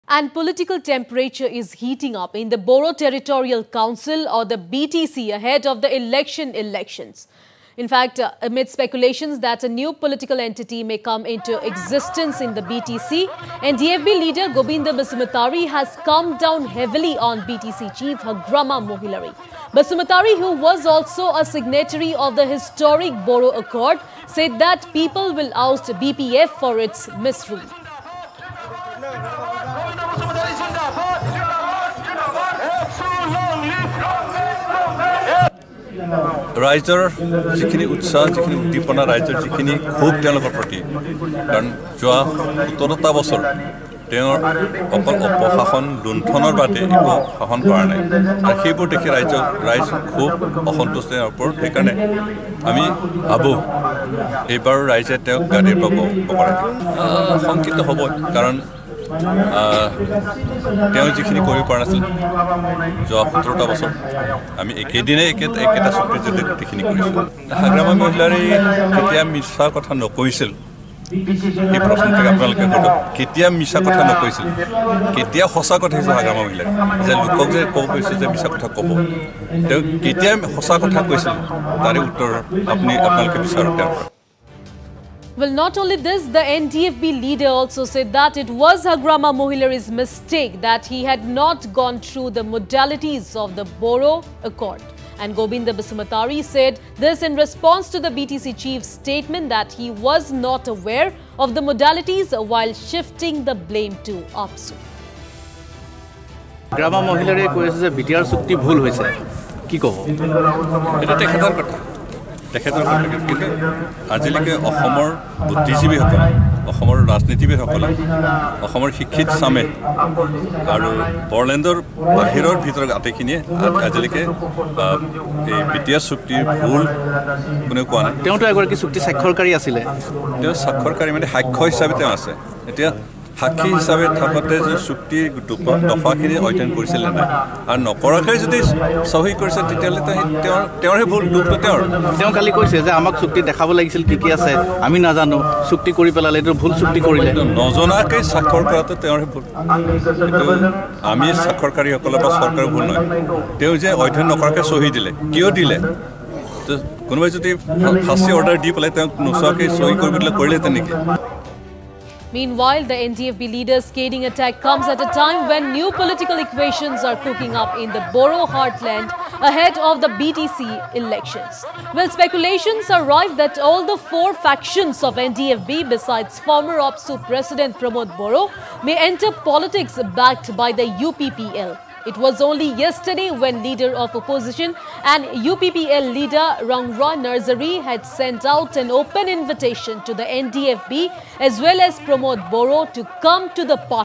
NDFB leader Gobindo Basumatary comes down heavily on BTC Chief Hagrama Mohilary
Political temperature is heating up in the Bodo Territorial Council or BTC ahead of the council elections.
Amidst speculations that a new political entity may come into existence in the BTC, NDFB leader Gobindo Basumatary has come down heavily on BTC Chief Hagrama Mohilary.
The NDFB leader also said that it was Hagrama Mohilary's mistake that he had not gone through the modalities of the Bodo Accord.